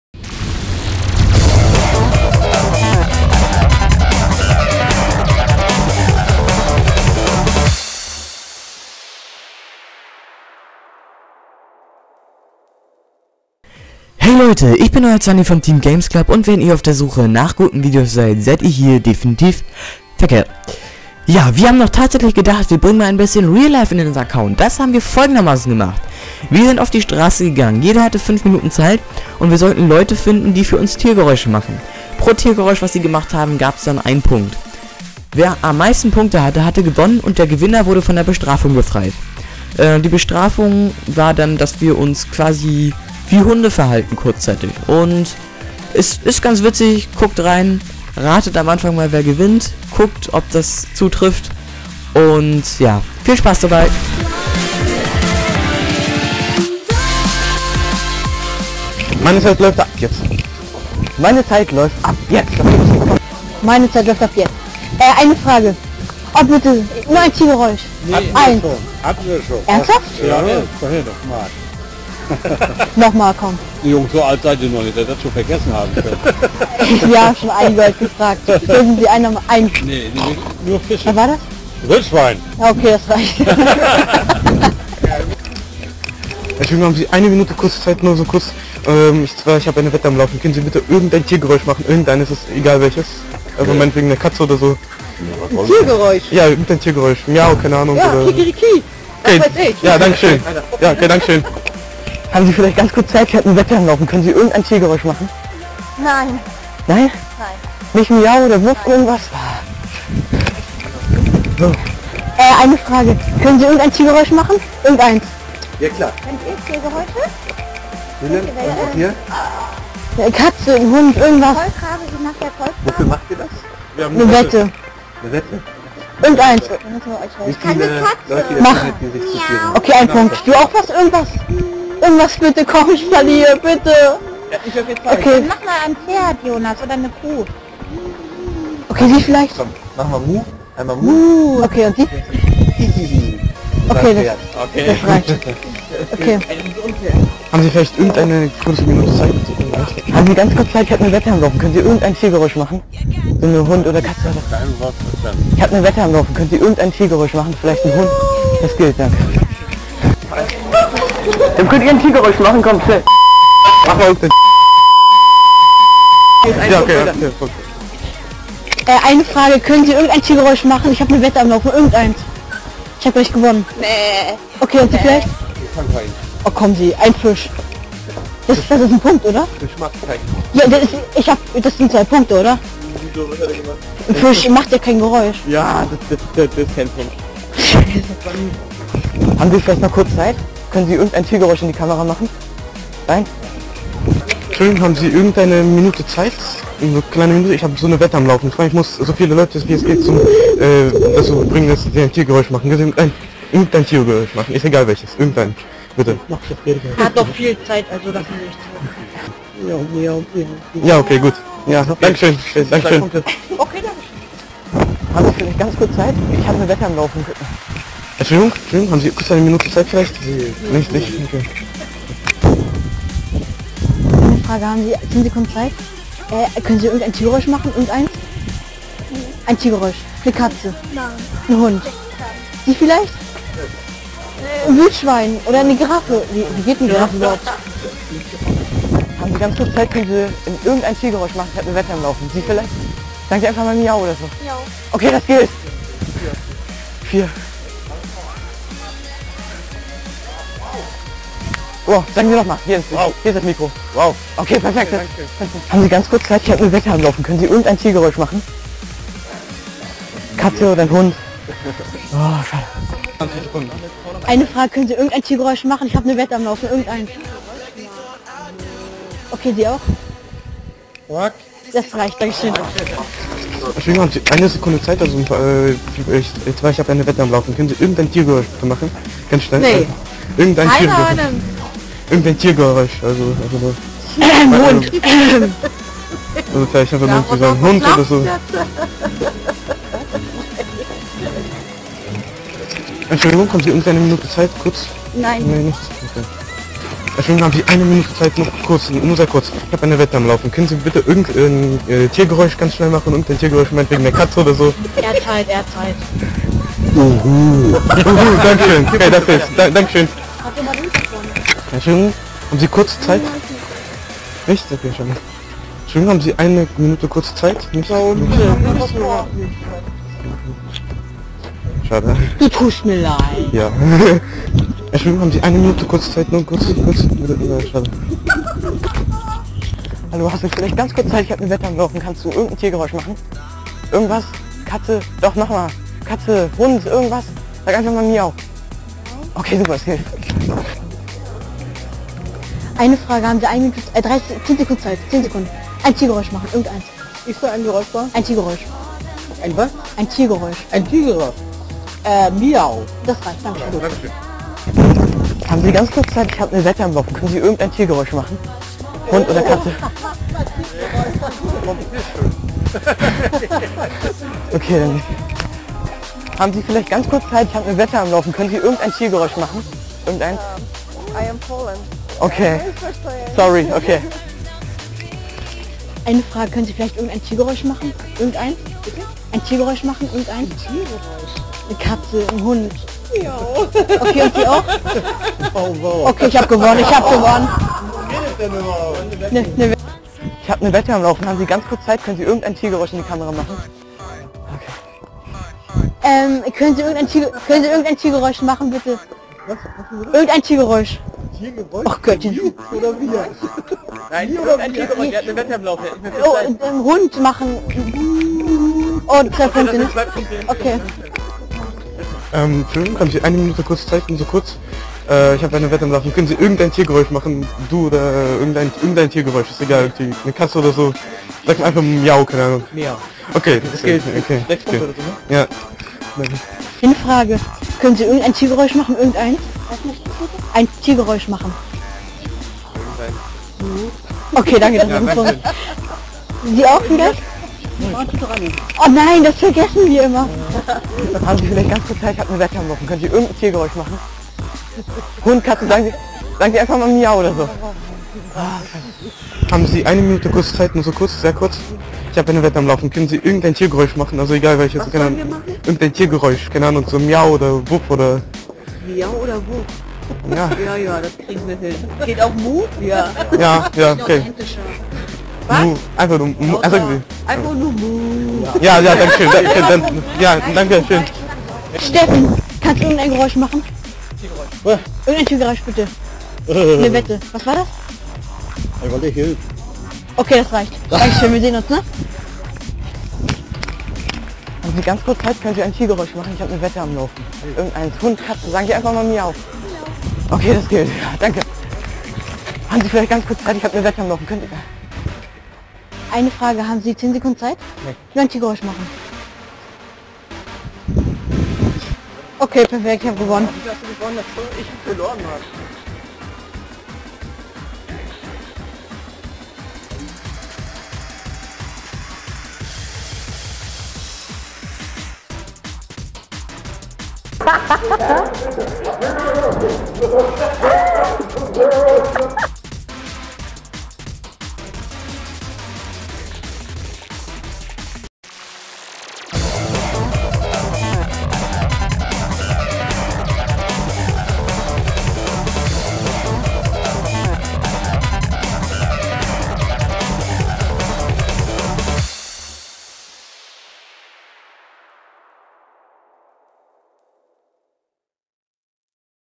Reallife Battle - Tier Geräusche
Das erste Street Battle Video ist draußen. Zanni,Jazz und Random King versuchen o viele mögliche Tiergeräusche von anderen leuten zu bekommen.